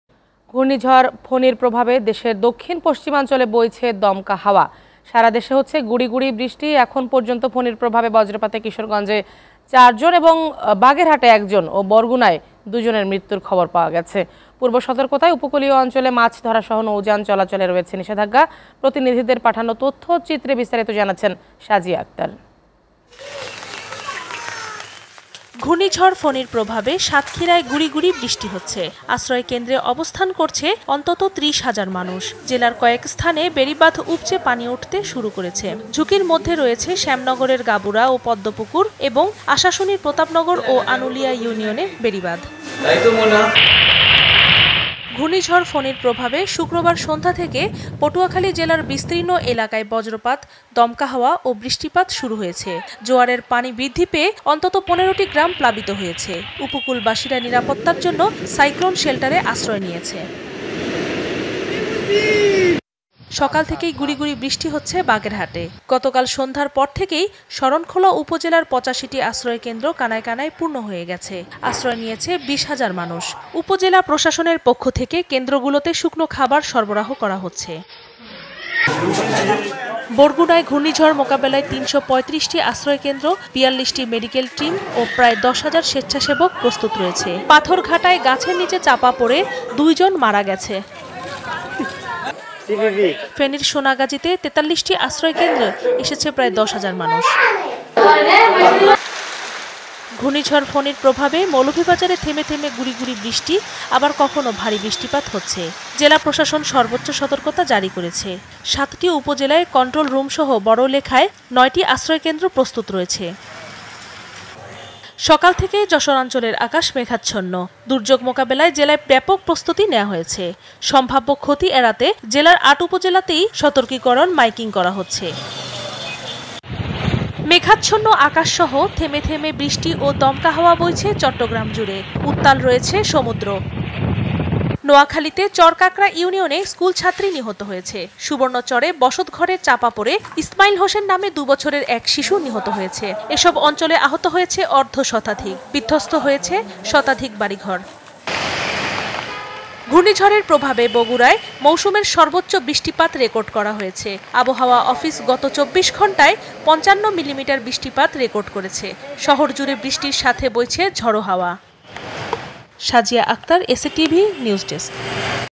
ঘূর্ণীঝড় ফণীর প্রভাবে দেশের দক্ষিণ-পশ্চিমাঞ্চলে বইছে দমকা হাওয়া | SATV News
SA TV Location:
Bangladesh